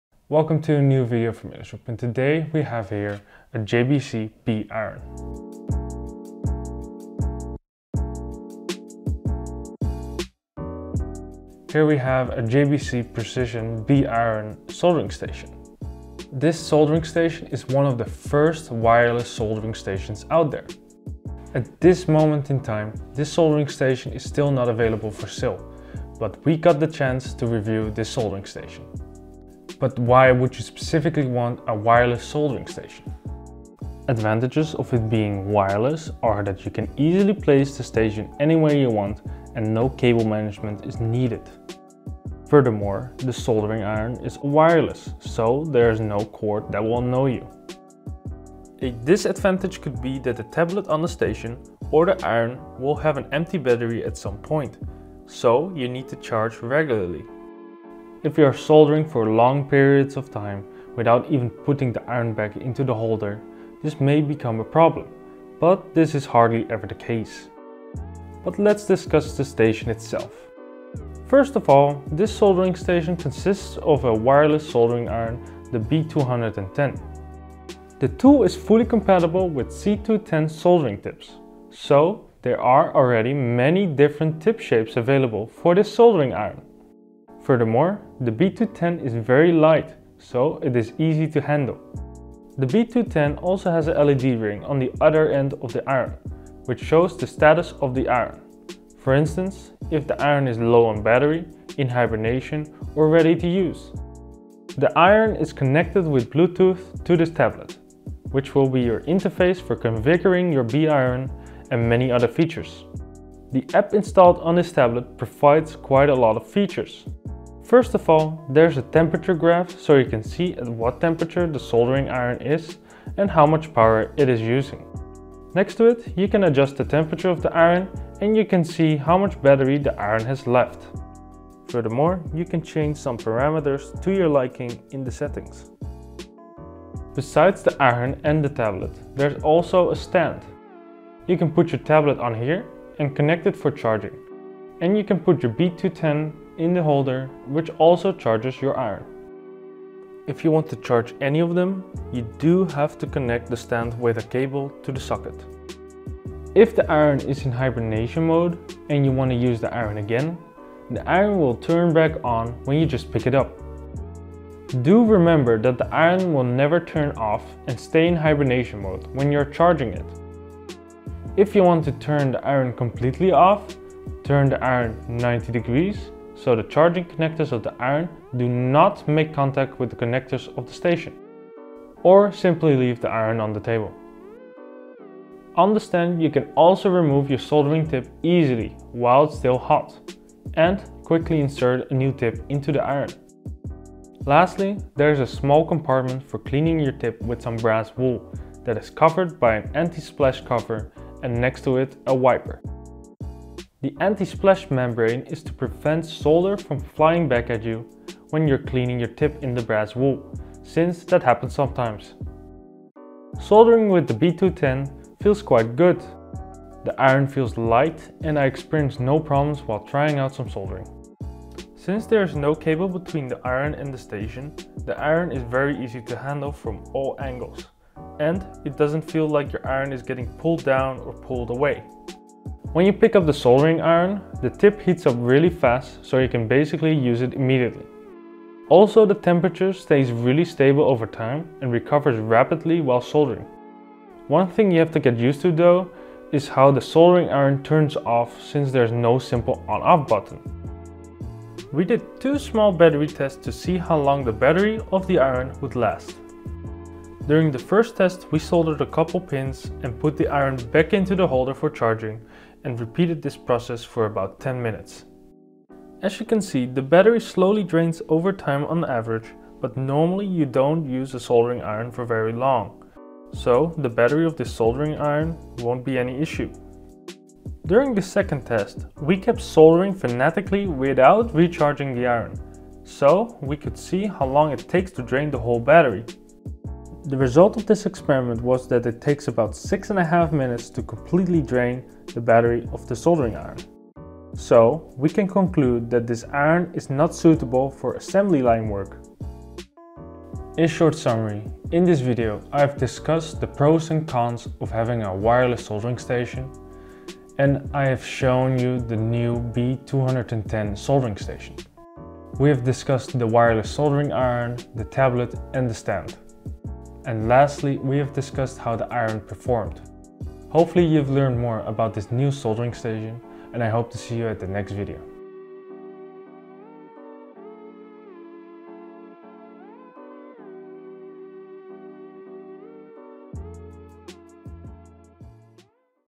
The new wireless JBC B-iRON soldering station
𝐓𝐡𝐞 𝐧𝐞𝐰 𝐉𝐁𝐂 𝐁-𝐢𝐑𝐎𝐍 𝐬𝐨𝐥𝐝𝐞𝐫𝐢𝐧𝐠 𝐬𝐭𝐚𝐭𝐢𝐨𝐧

In this video, Bas tells you everything you need to know about the new wireless JBC B-iRON soldering station. The soldering iron, the stand, and a tablet have been completely discussed on their features and how it feels during soldering. Furthermore, two small tests were done to check the battery life of the iron. Lastly, we give our opinion about this soldering station.